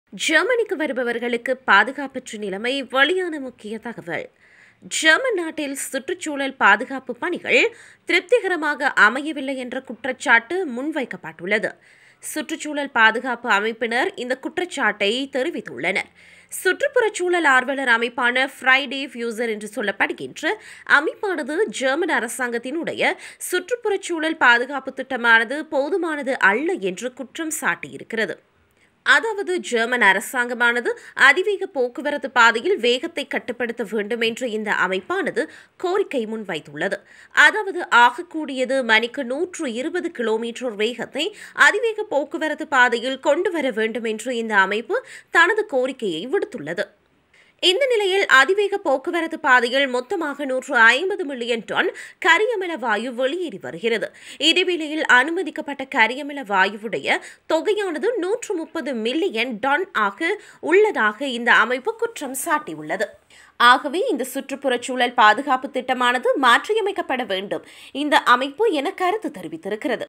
ஜெர்மனிக்கு வருபவர்களுக்கு பாதுகாப்பாற்ற நிலைமை - வெளியான முக்கிய தகவல்
ஜெர்மனி நாட்டில் சுற்றுச்சூழல் பாதுகாப்பு பணிகள் திருப்திகரமாக அமையவில்லை என்ற குற்றசாட்டு முன்வைக்கப்பட்டுள்ளது.